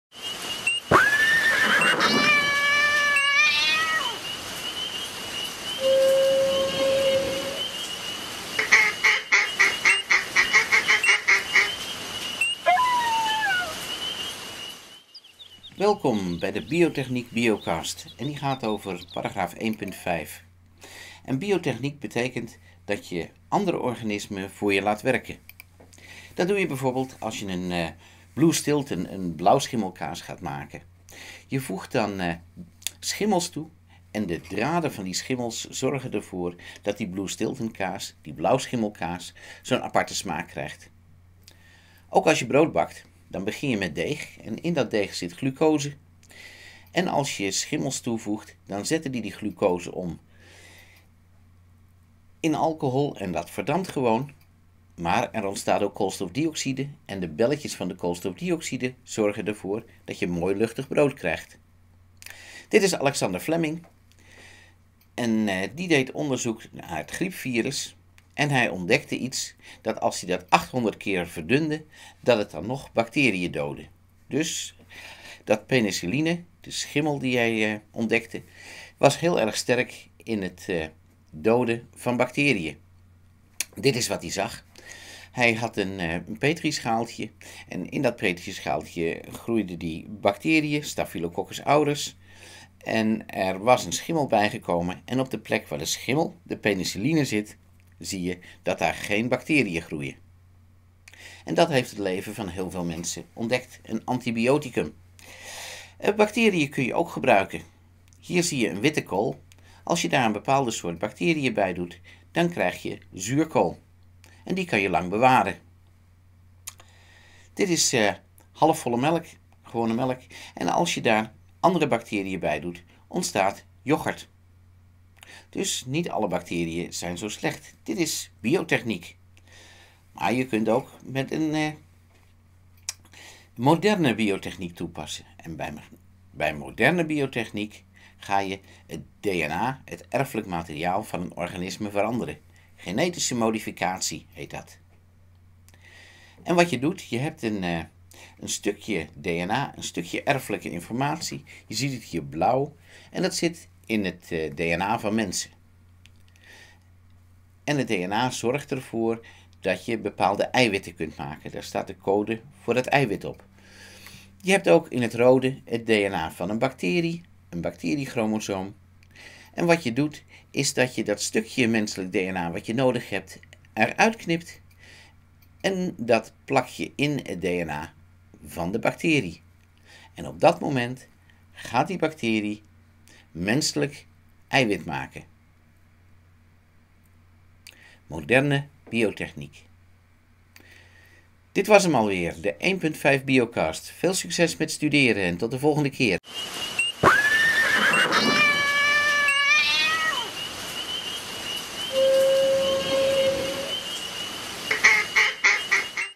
T 1.5 biocast
Een podcast over hoofdstuk 1.5 Nectar voor HKK 3 (TL)